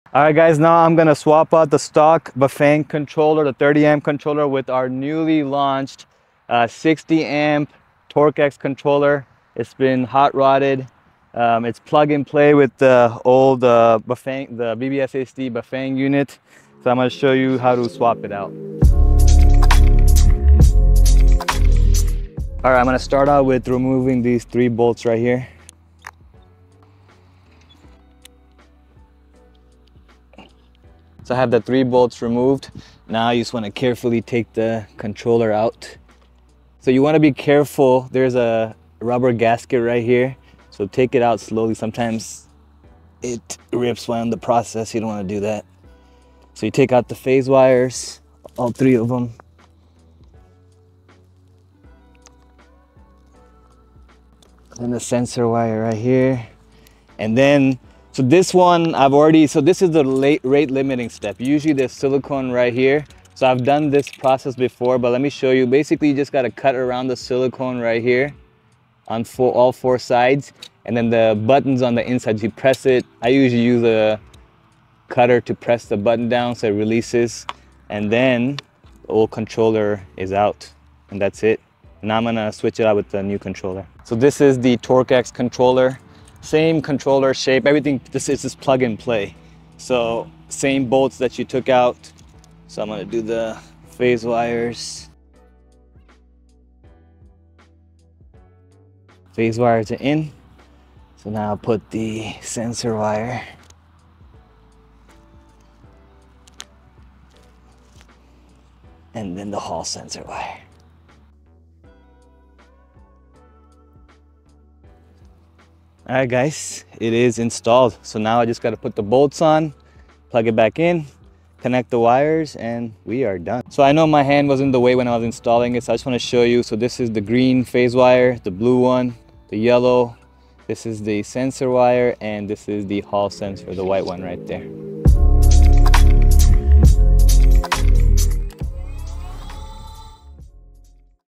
Upgrade BBSHD to 3000 Watts!! Plug and Play Controller
Plug and play 3000 Watt Bafang BBSHD TorqueX controller. Video how to change your stock controller to a upgraded controller. Riding Footage coming soon!